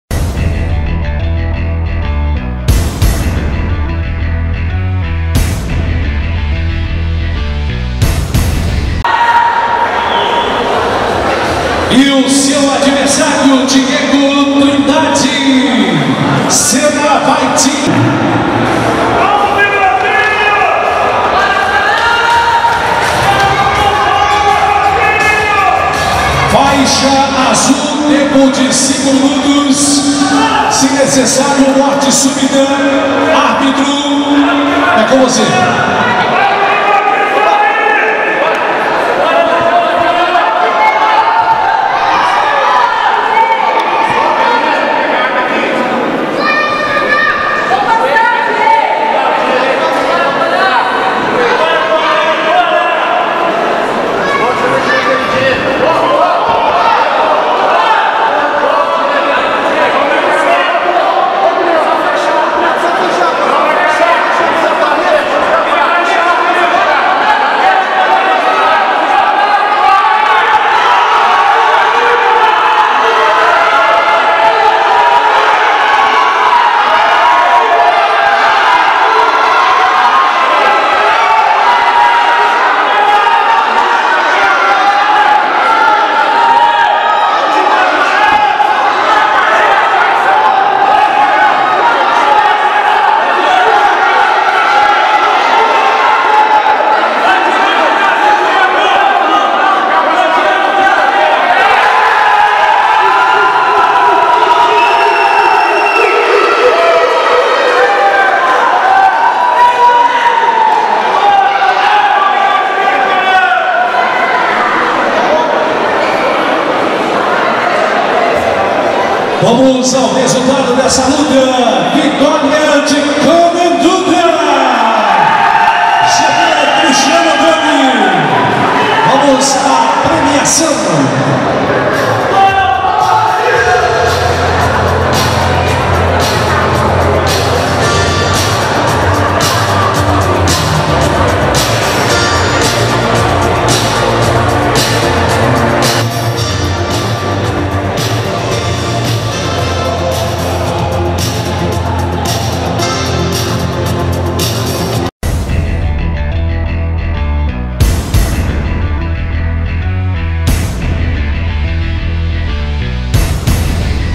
Diego vs Konan - Open Fight Championship 2
Em combate de Jiu-Jitsu até 80kg faixa azul, Konan Dutra (GB Cristiano Otoni) venceu Diego Trindade (Sena Fight Team) por finalização.